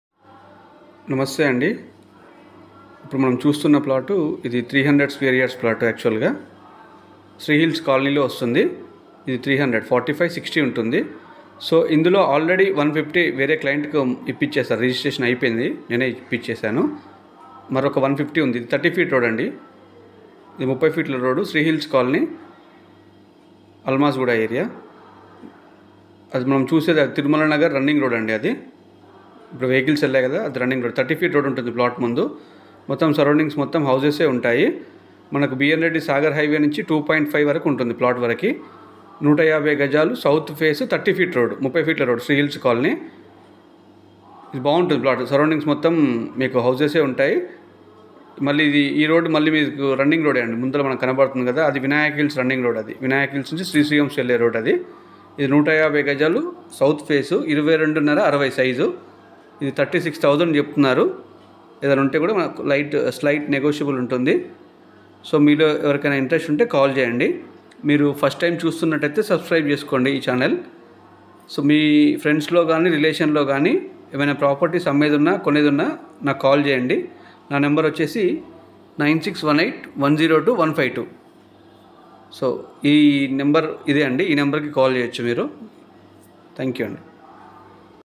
Open plot for SALE@ ALAMASGUDA, SRIHILLS COLONY, 150 yards, 30 feetroad. 2.5km BNreddy Sagar highway
Open plot for Sale@ SRIHILLS colony, alamasguda area..
near BNreddy nagar..
2.5 km from BNreddy nagar Sagar highway

150 sq yards..
South face..30 feet road..
22.6*60 size..
LRS token paid..

near to Bharathi vidayala,school, near to Arka international school..near to santinos school, krishnaveni talent school.
near metro hospital..

price 36000 slightly negotiable..

more details
Krishna Rao